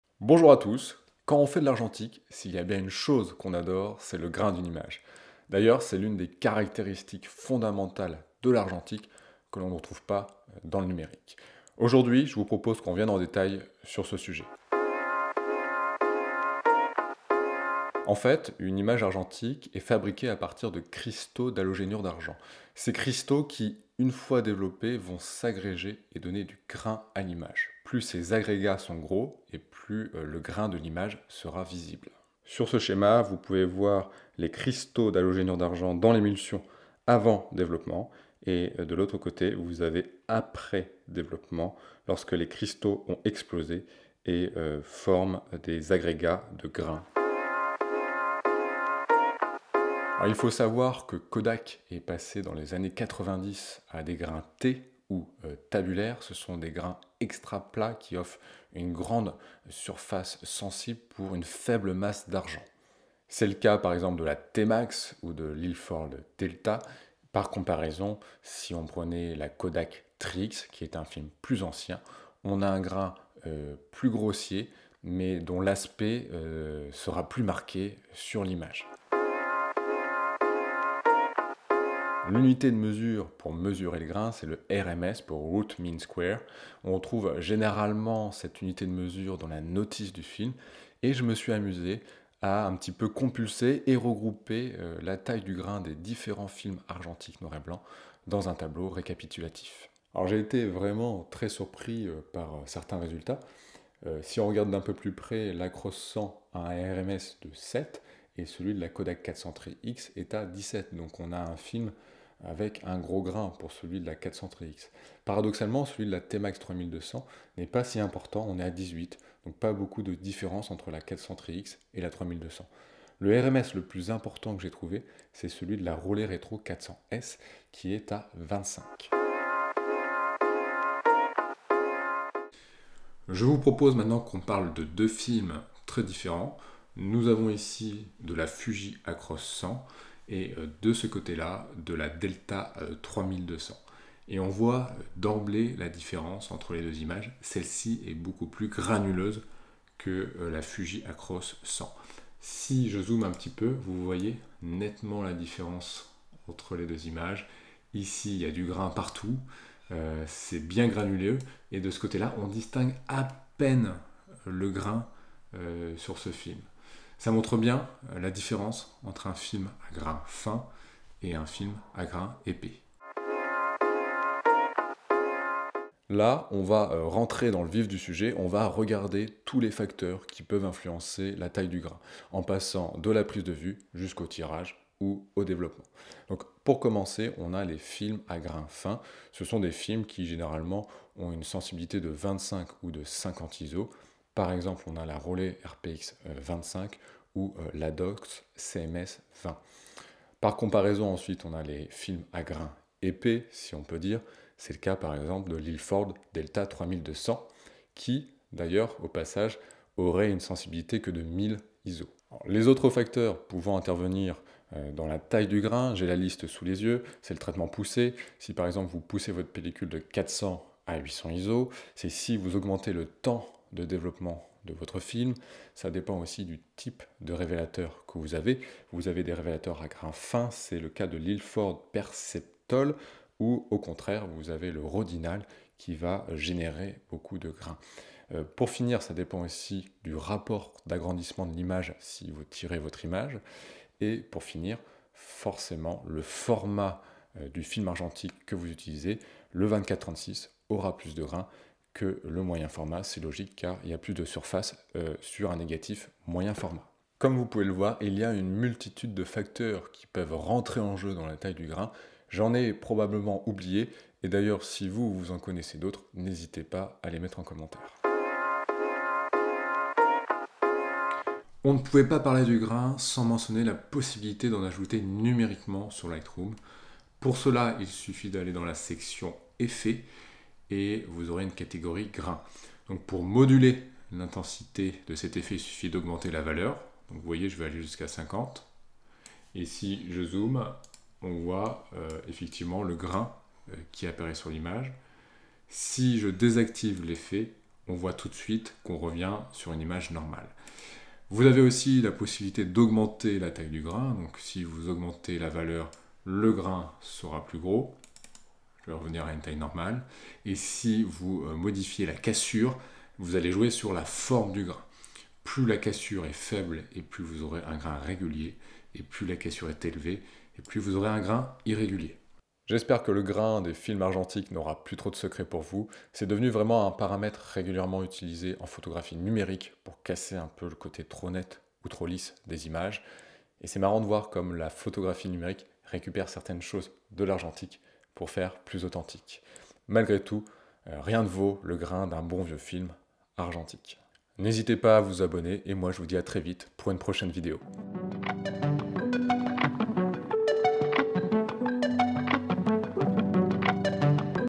Les secrets du grain en argentique !
Au programme :
- L'origine du grain (cristaux d'halogénures d'argent)
- Les formes de grain (tabulaire)
- Comparaison du grain entre 2 films
- Les facteurs influençant le grain (type de film, Rodinal ect..)
- Ajout du grain numériquement avec Lightroom